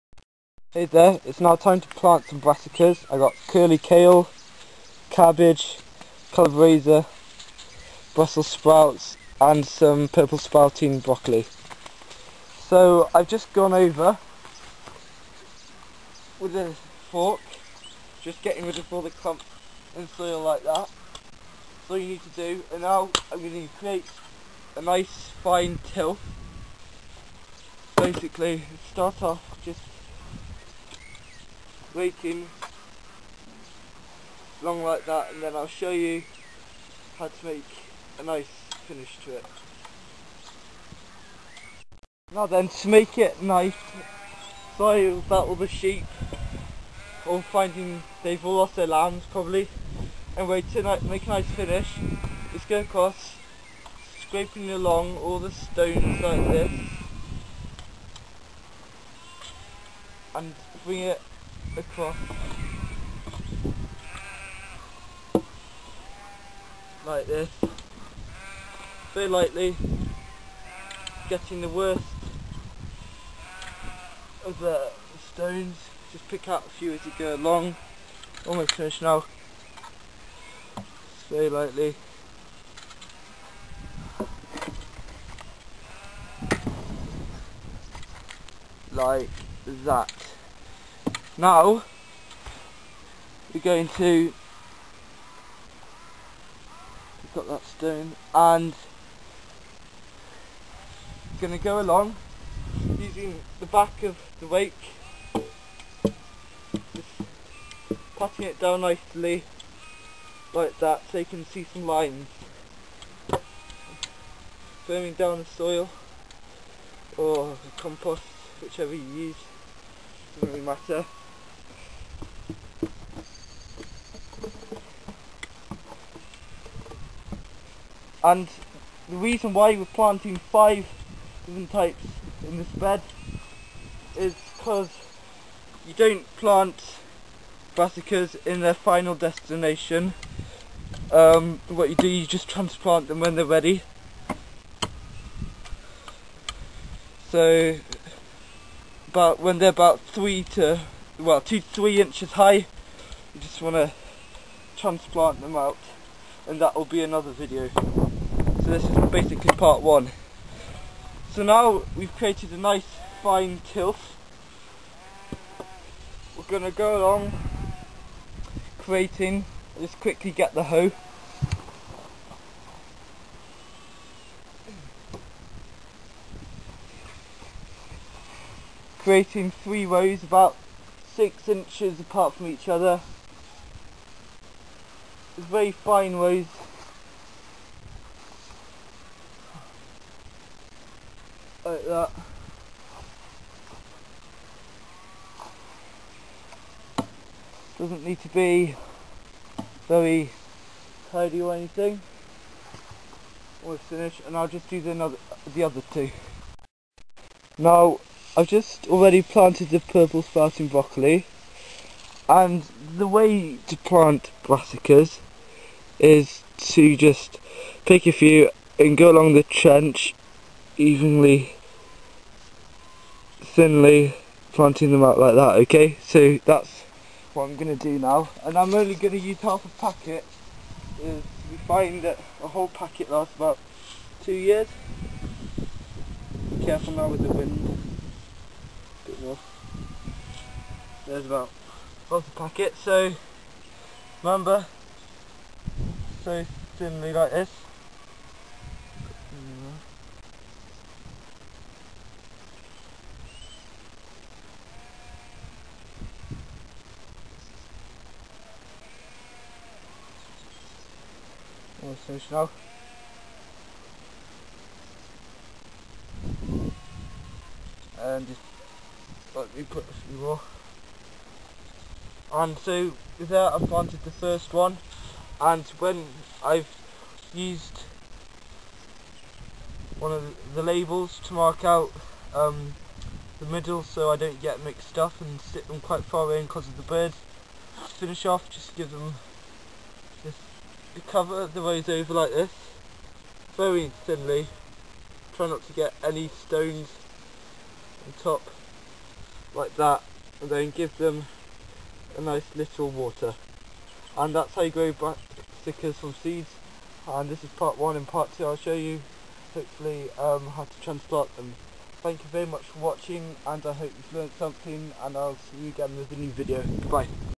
How to grow Cabbages, Sprouts and Kale from Seed
Cabbage, Brussel sprouts and kale are all types of brassicas. Here I show you how to grow them from seed.